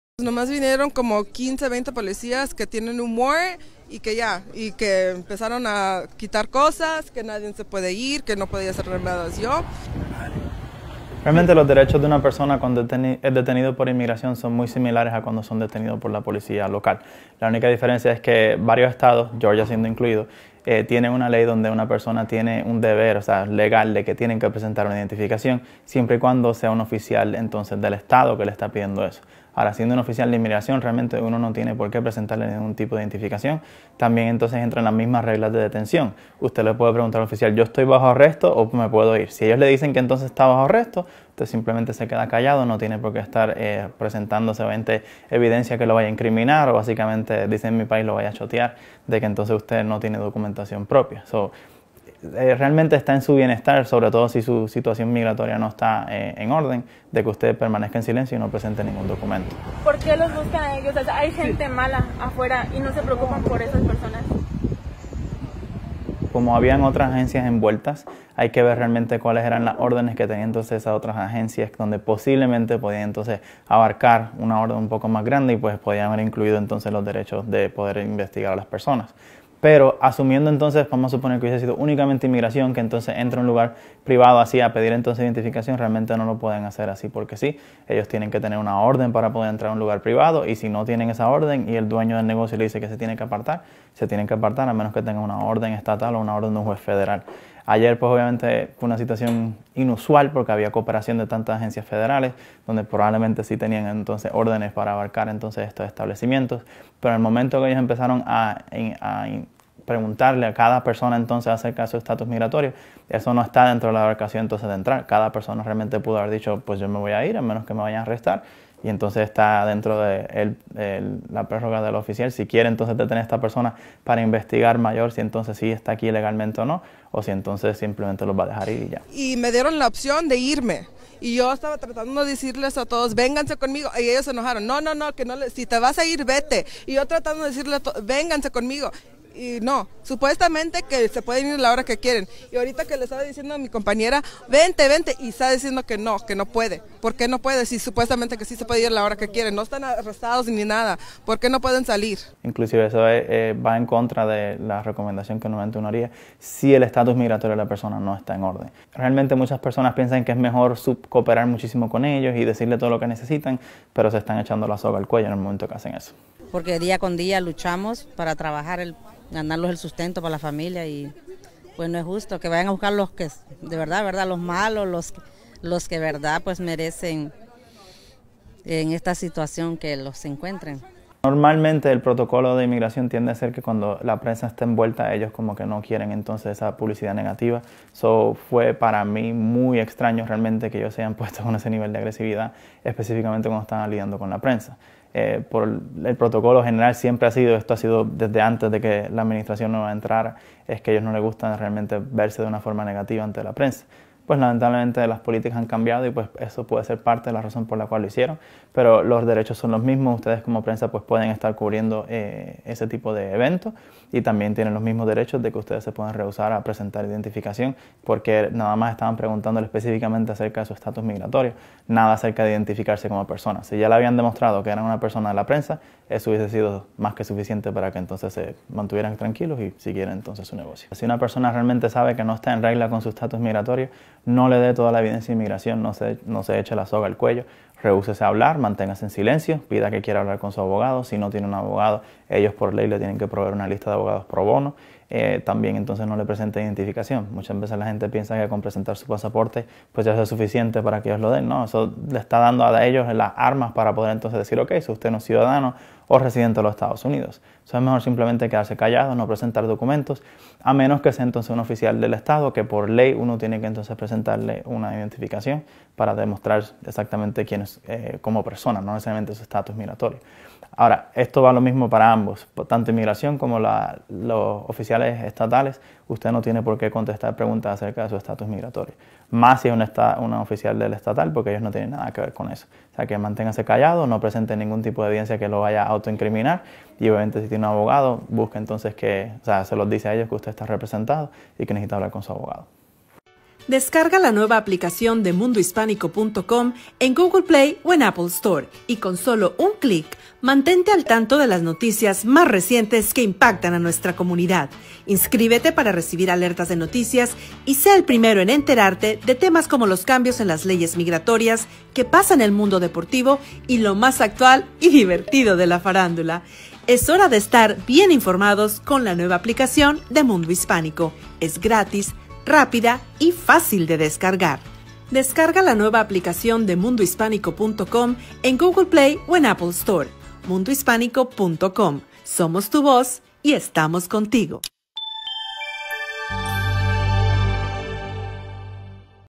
Recomendaciones en caso de estar en una redada de ICE
¿Qué hacer ante una situación similar? El abogado Michael Urbina ofrece una serie de recomendaciones para mantener a los inmigrantes en alerta y así enfrentar una posible intervención.

* El inmigrante puede pedir hablar con su abogado si no tiene un abogado, ellos, por ley tienen, que proveer una lista de abogados.

* No tiene que contestar preguntas acerca de su estatus migratorio si es un oficial de la estatal, porque ellos no tienen nada que ver con eso, por lo tanto manténgase callado y no presente ningún tipo de evidencia que lo vaya a incriminar.